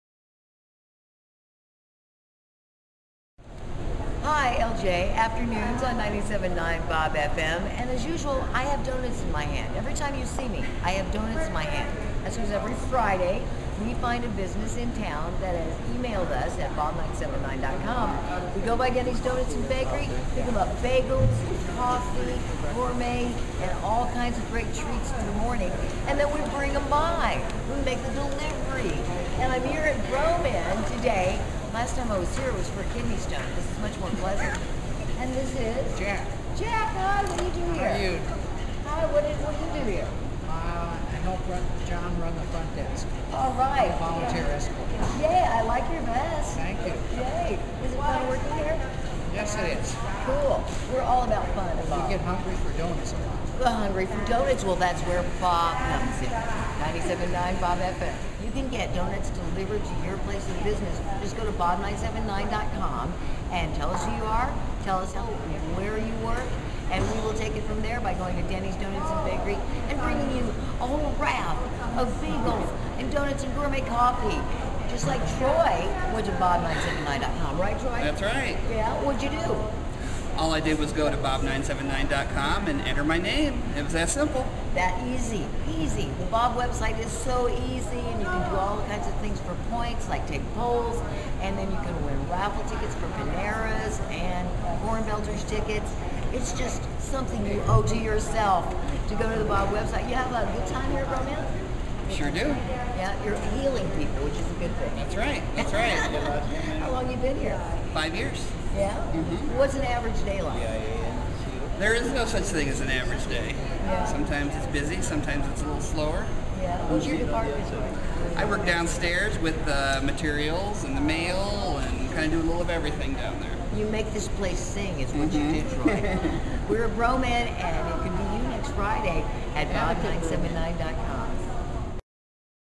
BobDonutBroMenn.wmv
The BOB Squad pays a visit to Troy at BroMenn Regional Medical Center.  Troy registered for and won BOB Buys Breakfast!